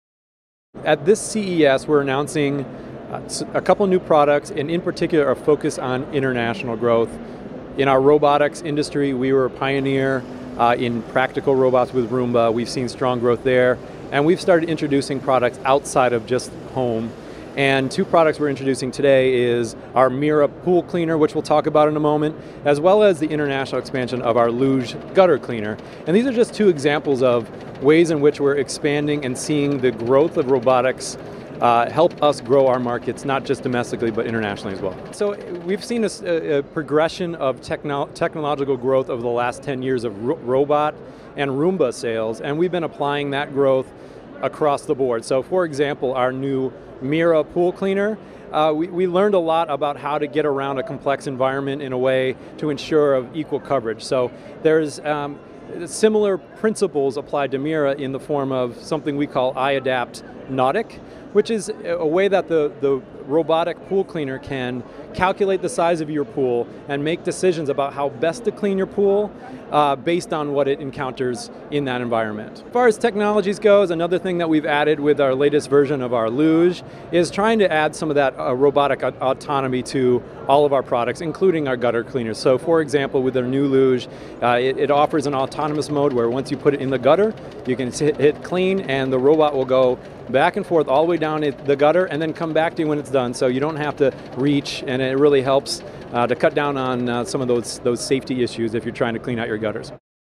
Where is iRobot Navigating Next?
iRobot's (Nasdaq: IRBT) stock has underperformed the market since its 2005 IPO. Investors are hoping things turn around, however, as the company builds on its existing cleaning and military robotic technology and branches into new products -- such as the RP-VITA, now cleared for use in hospitals.

Motley Fool analyst Rex Moore met with iRobot at the recent consumer electronics show in Las Vegas, and asked about future growth areas for the company.